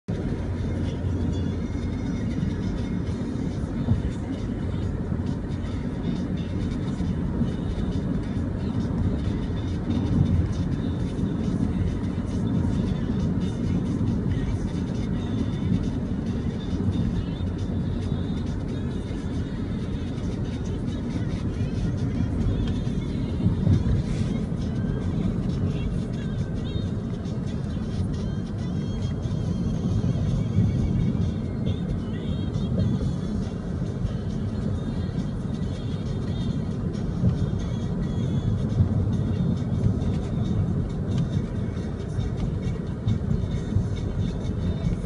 Пожар в п.Садовый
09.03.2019 г. горят дачные постройки в дачном поселке Садовый Дальнегорского городского округа.